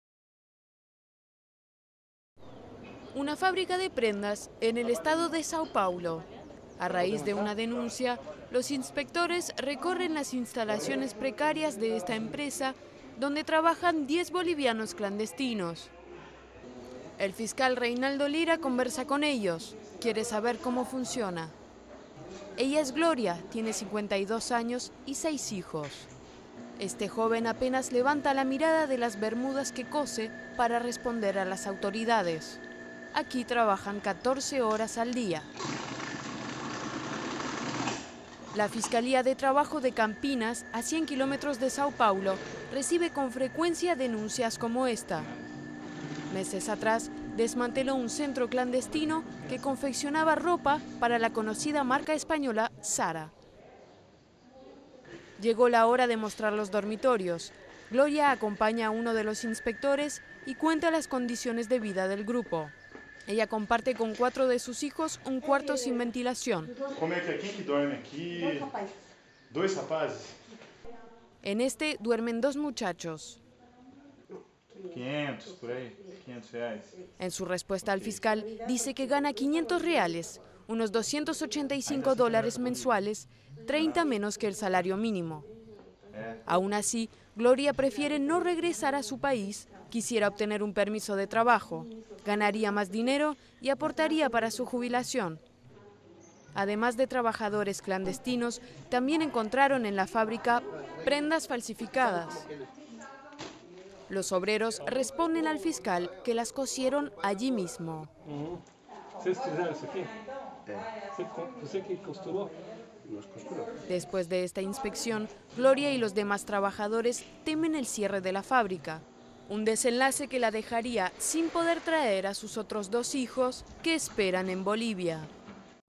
Bolivianos esclavizados en Brasil
Muchos bolivianos llegan a Sao Paulo en busca de oportunidades. Tienen niños pequeños, no hablan portugués. El trabajo para ellos suele ser inhumano y mal pago. A pesar de todo, optan por estos talleres de costura en lugar de regresar a su país. Un informe de AFPTV.